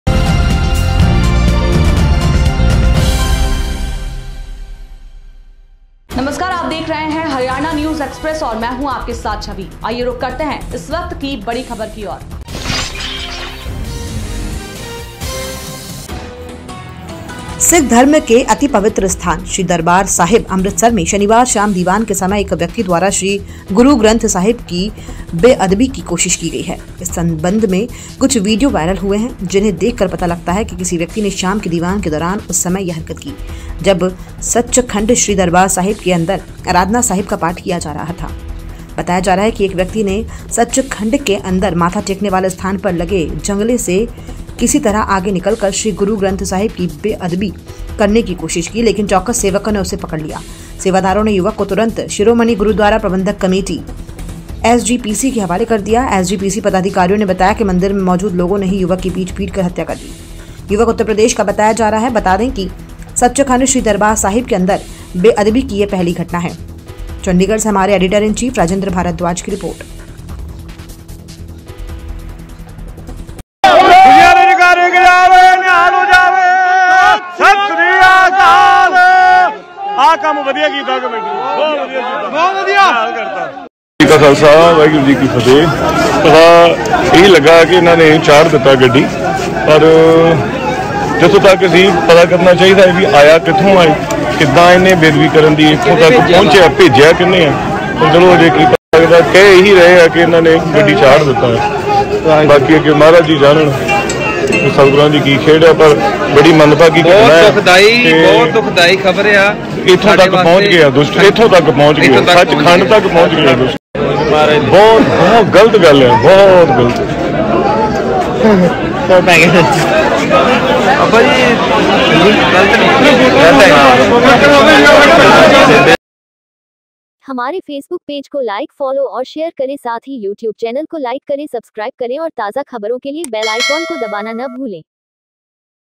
स्वर्ण मंदिर में श्री गुरु ग्रंथ साहिब की बेअदबी की कोशिश, लोगों ने आरोपी को पीट-पीटकर मार डाला
सिख धर्म के अति पवित्र स्थान श्री दरबार साहिब, अमृतसर में शनिवार शाम दीवान के समय एक व्यक्ति द्वारा श्री गुरु ग्रंथ साहिब की बेअदबी की कोशिश की गई है। इस संबंध में कुछ वीडियो वायरल हुए हैं, जिन्हें देख कर पता लगता है कि किसी व्यक्ति ने शाम के दीवान के दौरान उस समय यह हरकत की, जब सच्चखंड श्री दरबार साहिब के अंदर आराधना साहिब का पाठ किया जा रहा था। बताया जा रहा है कि एक व्यक्ति ने सच्चखंड के अंदर माथा टेकने वाले स्थान पर लगे जंगले से किसी तरह आगे निकल कर श्री गुरु ग्रंथ साहिब की बेअदबी करने की कोशिश की, लेकिन चौकस सेवकों ने उसे पकड़ लिया। सेवादारों ने युवक को तुरंत शिरोमणि गुरुद्वारा प्रबंधक कमेटी एसजीपीसी के हवाले कर दिया। एसजीपीसी पदाधिकारियों ने बताया कि मंदिर में मौजूद लोगों ने ही युवक की पीट-पीटकर हत्या कर दी। युवक उत्तर प्रदेश का बताया जा रहा है। बता दें, सच्चखंड श्री दरबार साहिब के अंदर बेअदबी की यह पहली घटना है।